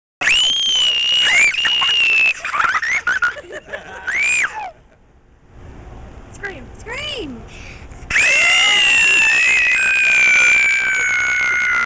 SCREAM
Sue and Dawn's girls show off their set of lungs!